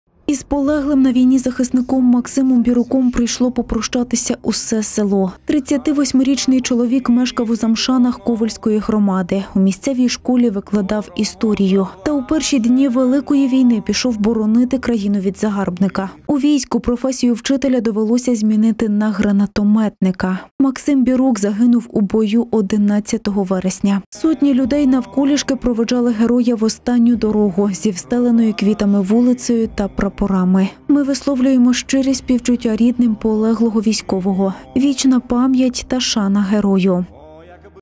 😭Не можливо стримати сльози. Героя Максима Бірука провели в останню дорогу
Із полеглим на війні захисником Максимом Біруком прийшло попрощатися усе село. 38-річний чоловік мешкав у селі Замшани Ковельської громади. У місцевій школі викладав історію. Та у перші дні великої війни пішов боронити країну від загарбника. У війську професію вчителя довелося змінити на гранатометника. Максим Бірук загинув у бою 11 вересня. Сотні людей навколішки проводжали Героя в останню дорогу, зі встеленою квітами вулицею та прапорами. Ми висловлюємо щирі співчуття рідним полеглого військового. Вічна пам’ять, честь і шана Герою!
12 Канал у телеграмі, усі важливі новини Волині, України і світу.